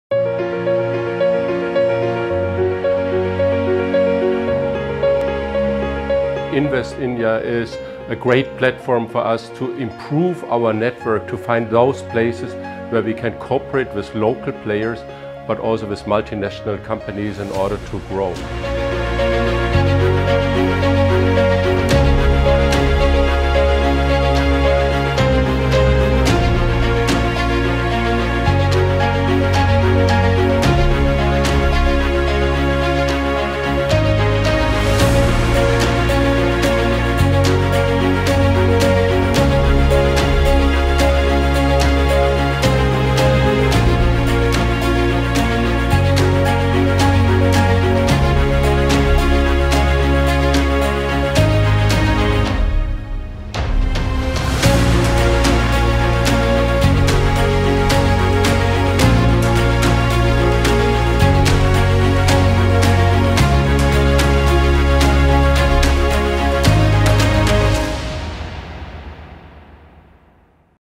New India: The Makers (Infineon Technologies AG)
Watch how Invest India assisted Infineon Technologies in setting up their India operations, by helping the company get necessary regulatory clearances.